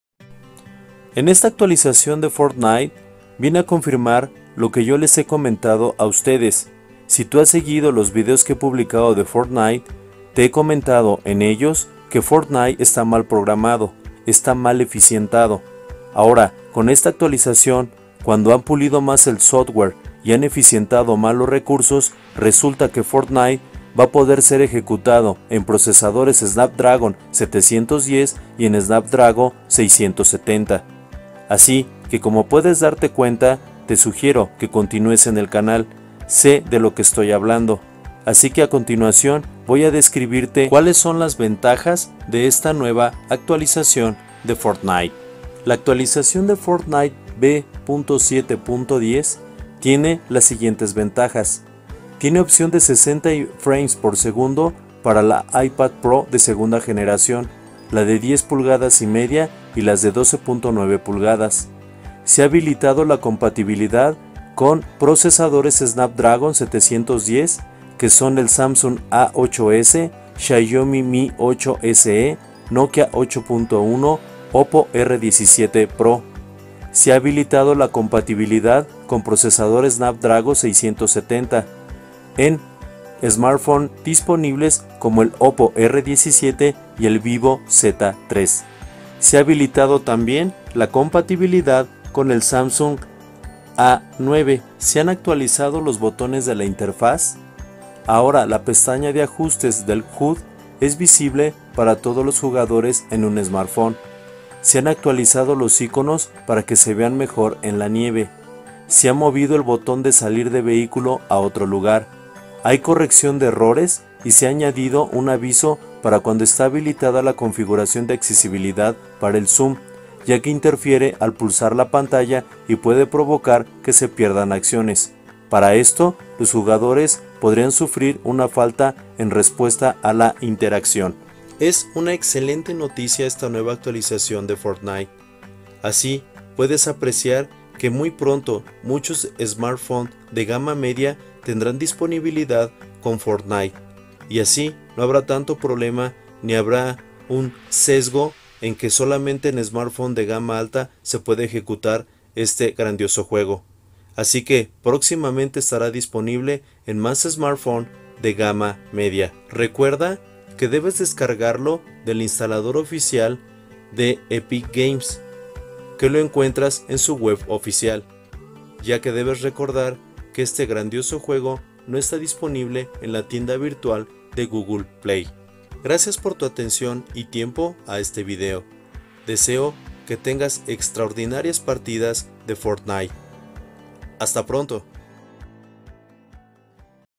FORTNITE YA ES COMPATIBLE CON SMARTPHONES DE GAMA MEDIA
La actualización por su séptima temporada, esta semana, los usuarios del título de Epic Games podrán disfrutar de las posibilidades de este battle royale en más Smartohones gracias a que se ha habilitado la compatibilidad con los microprocesadores Snapdragon 710 y Snapdragon 670.

Se trata de la actualizacion v.7.10 y tiene las siguientes caracteristicas:

Se ha habilitado la compatibilidad para los microprocesadores Snapdragon 710 como Samsung A8S, Xiaomi Mi 8SE, Nokia 8.1, Oppo R17 Pro.

Se ha habilitado la compatibilidad para los microprocesadores Snapdragon 670 como Oppo R17, Vivo Z3.

Se ha habilitado la compatibilidad con Samsung A9.

Se han actualizado los botones de la IU.

Ahora la pestaña de ajustes de HUD es visible para los jugadores de Smartphone.

Se han actualizado los iconos para que se vean mejor en la nieve.

Se ha movido el botón de salir del vehículo a otro lugar.

Corrección de errores: Se ha añadido un aviso para cuando está habilitada la configuración de accesibilidad para el zoom, ya que interfiere al pulsar la pantalla y puede provocar que se pierdan acciones. Por ello, los jugadores podrían sufrir una falta de respuesta en las interacciones.

Opción de 60 imágenes por segundo habilitada para iPad Pro de segunda generación 10,5″ y 12,9″ elegir en los ajustes.

MUY PRONTO EN MAS SMARTPHONES DE GAMA MEDIA.